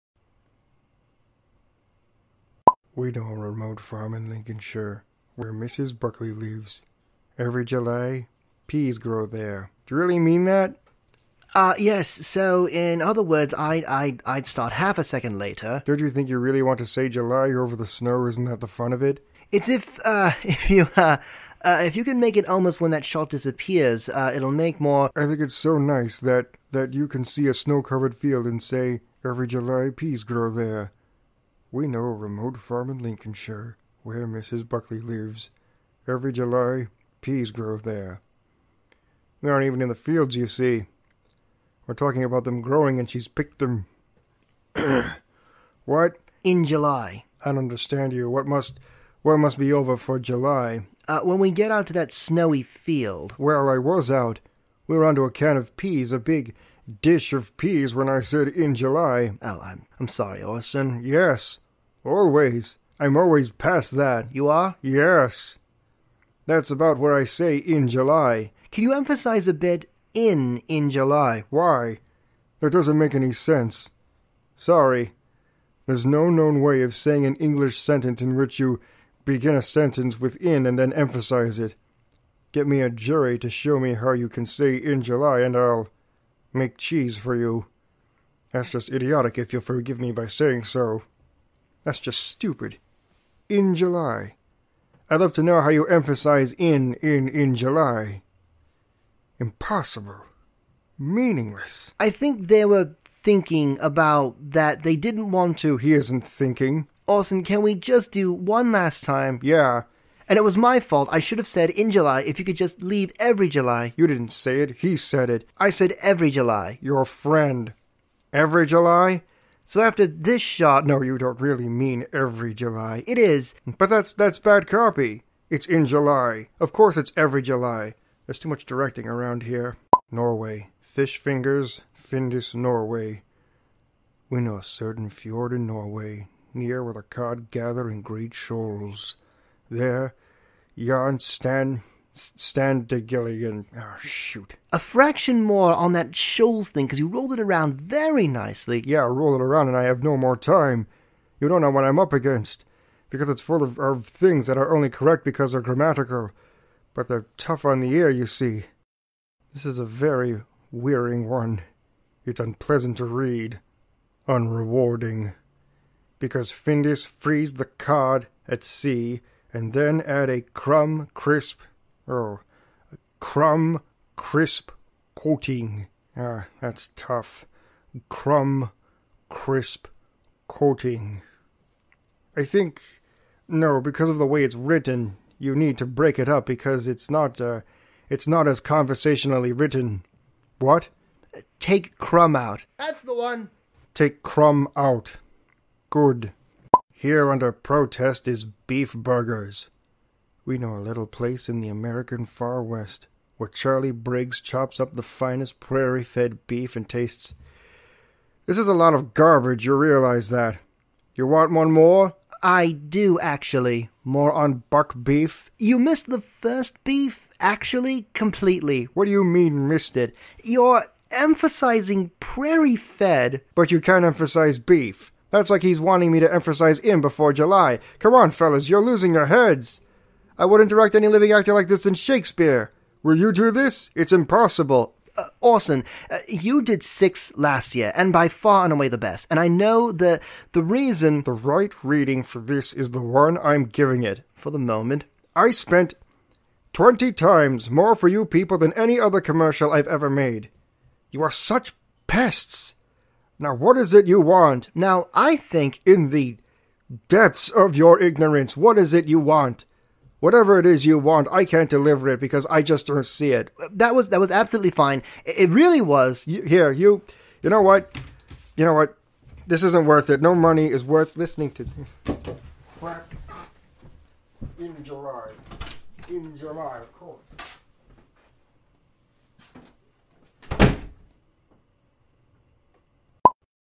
Orson Welles - Frozen Peas
A reading of the famous "frozen peas," recording of the late Orson Welles. With all due respect to one of the greats.

Voices by David Lanzafame

Images taken from respective sources.